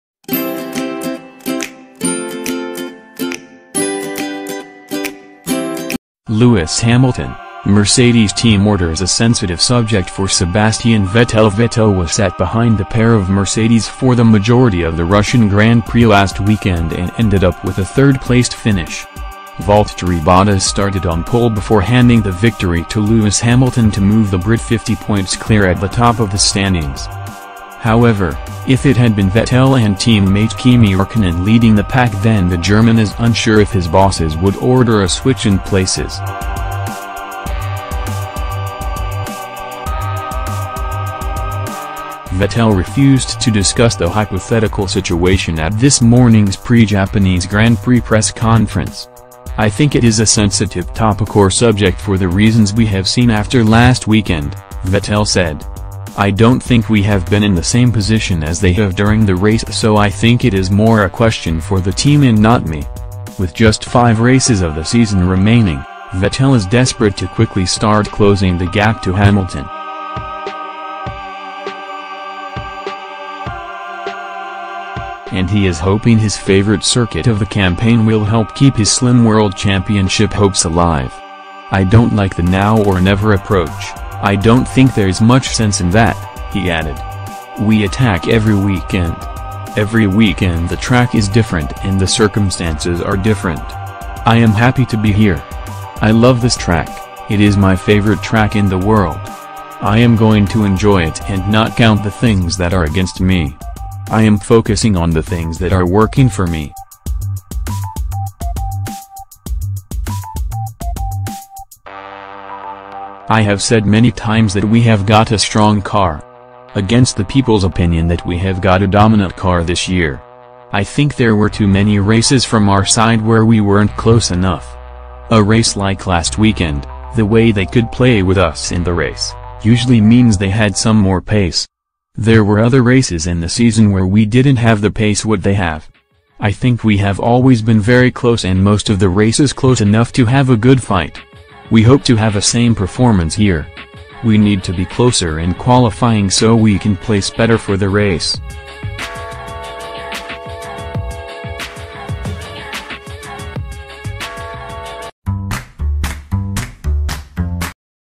Lewis Hamilton: Mercedes team orders a ‘sensitive subject’ for Sebastian Vettel
SEBASTIAN VETTEL says the difference between Mercedes and Ferrari’s team orders are a “sensitive subject.”